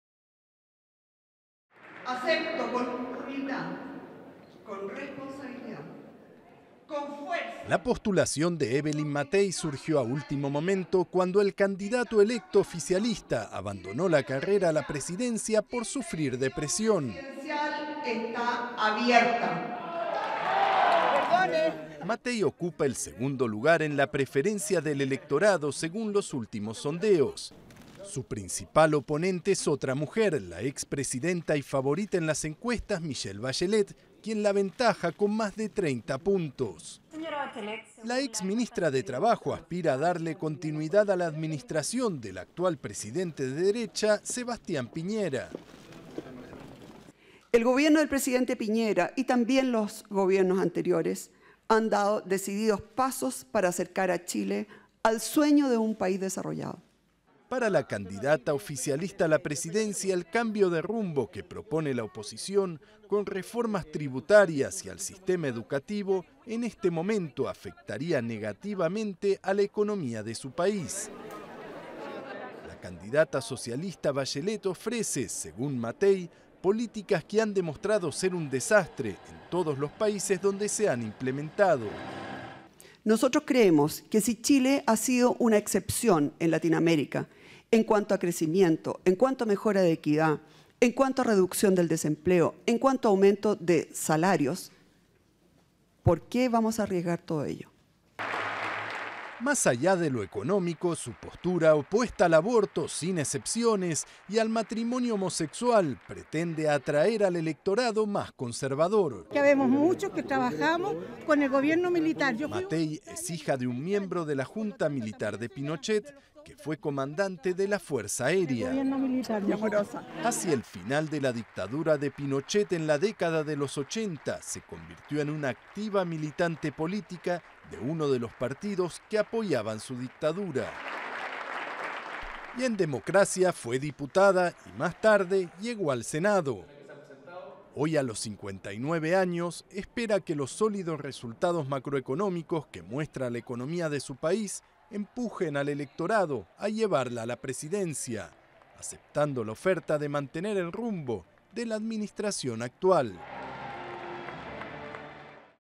Chile, la candidata Evelyn Matthei
La candidata oficialista, Evelyn Matthei, que ocupa un lejano segundo lugar en las encuestas intenta alcanzar a la candidata socialista, Michelle Bachelet, proponiendo una agenda continuista y conservadora.